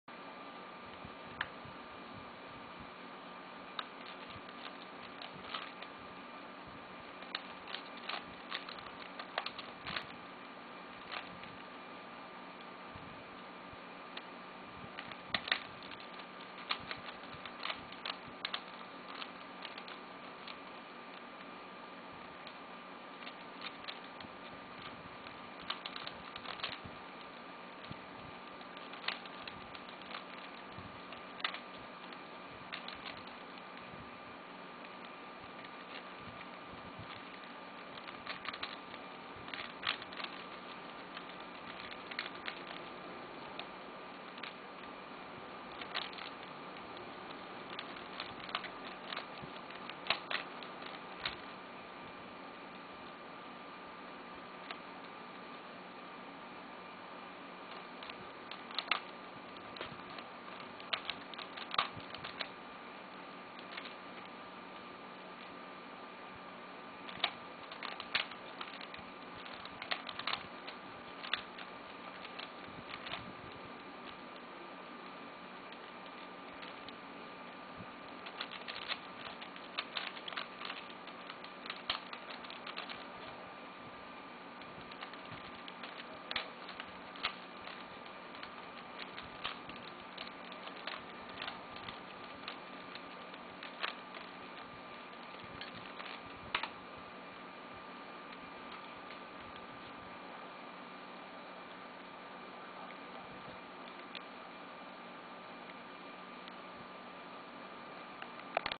Master Pyramorphinx
My Master Pyramorphinx solve
Normally I solve it in about 2 min.
No smart method, just beginner's method.

解四色粽子魔術方塊 (meffert)